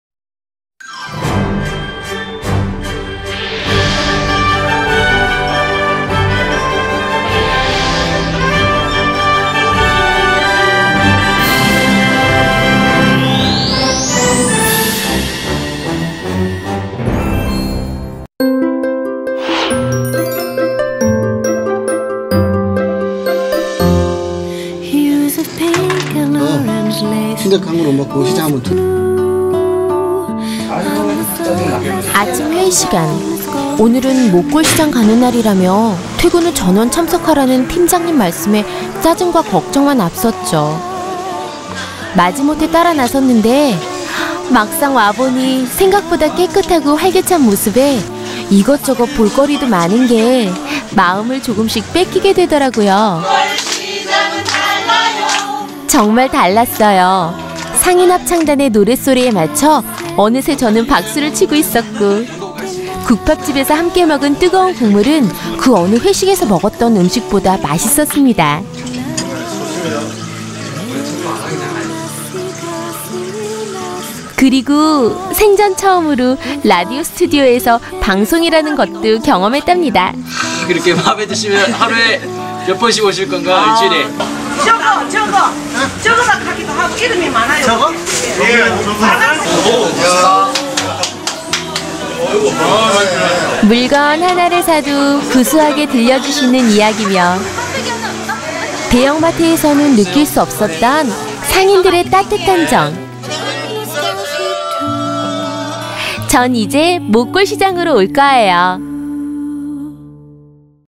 KT 미녀사원 오정희씨의 전통시장 이야기
KT 수원 전화국에 근무하는 오정희씨.
퇴근 후 전통시장 장보기 행사에 전원 참석하라는 팀장님의 지시에 아침부터 짜증이 납니다.
하지만 전통시장을 찾은 그녀는 어느새 박수를 치고, 웃고 있었는데...
그녀에게 어떤 일들이 벌어진걸까요?